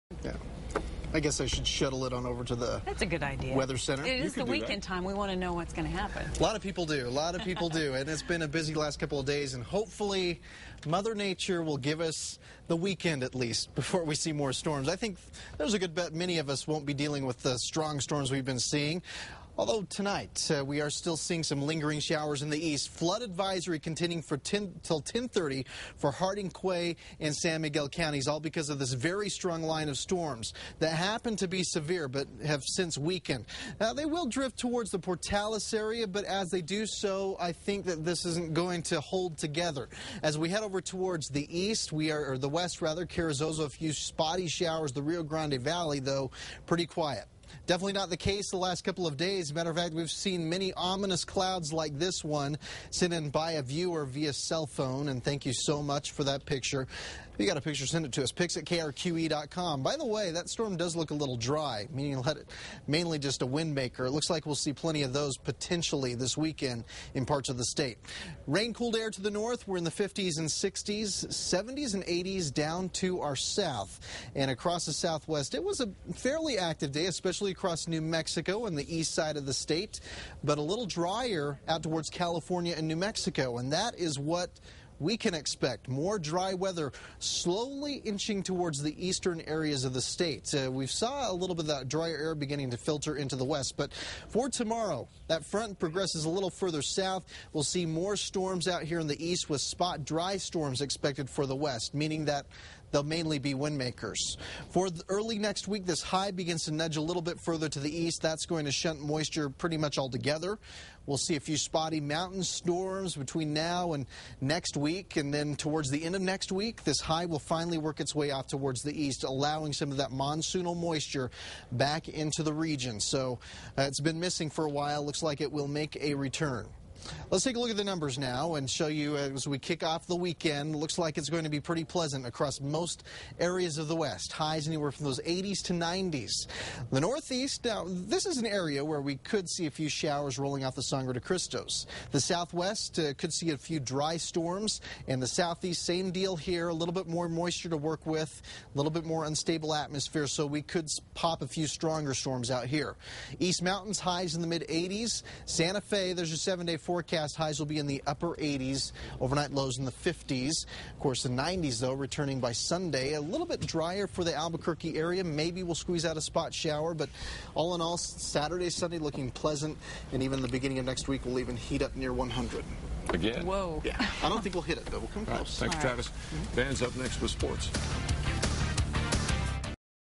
Overnight forecast July 31, 2009
KRQE Meteorologist Travis Christy's 10 p.m. weathercast.